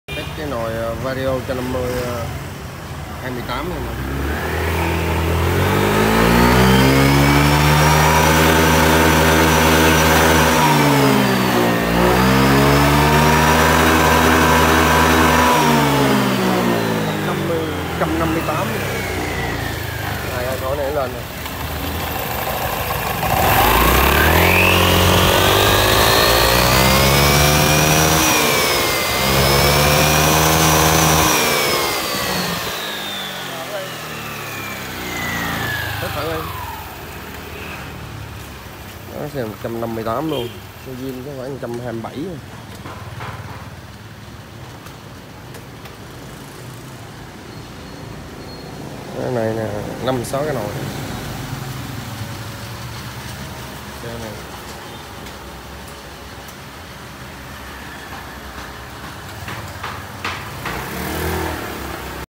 Vario 150i làm nồi chạy vọt êm ái test lên 158 km/h giúp xe tiết kiệm xăng tăng vòng tua máy tại TB
Vario 150i làm nồi chạy vọt êm ái test lên 158 km/h giúp xe tiết kiệm xăng tăng vòng tua máy tại TBK Team Bảo Hello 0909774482